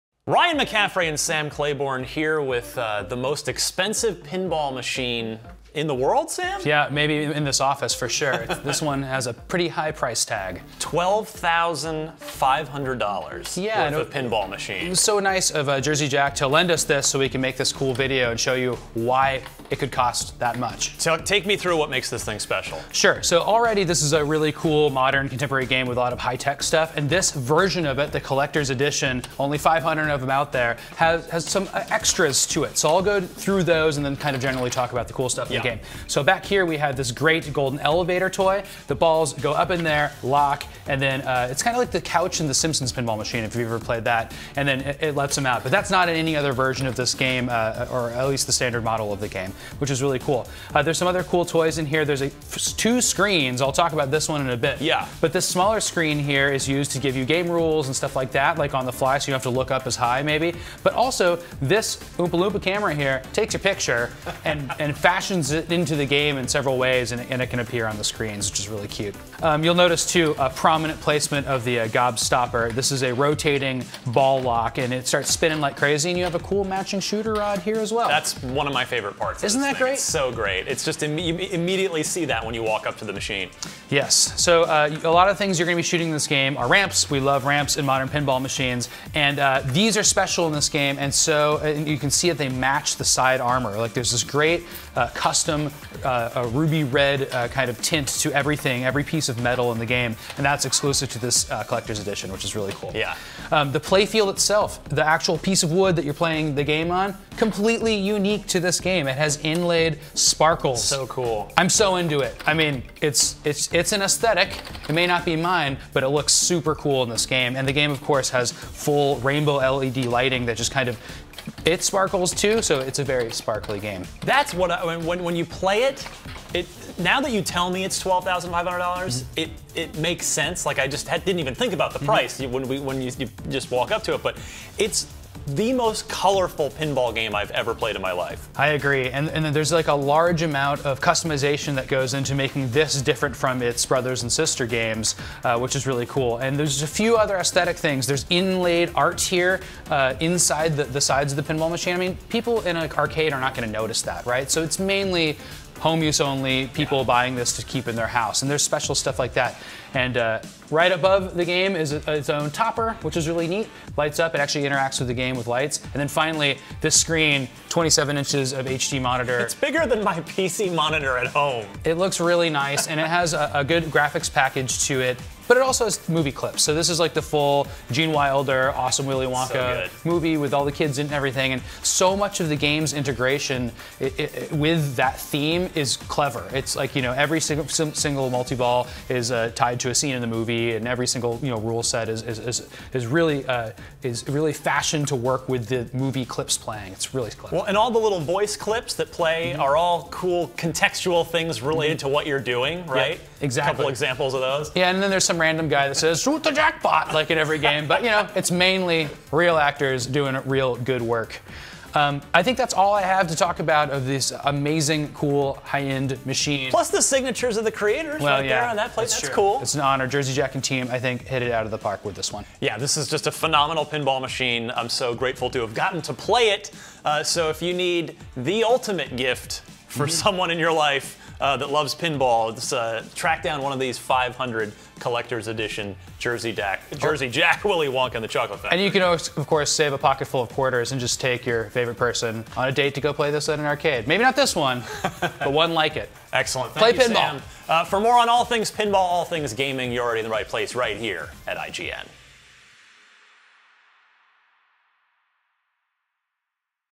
This Limited Edition Willy Wonka Pinball Machine May Be Most Expensive Ever, Here's Why
Jersey Jack Pinball's Willy Wonka Collector's Edition (CE) pinball machine may not show up in your local arcade. Here's a tour of its high tech, fancy gadgets.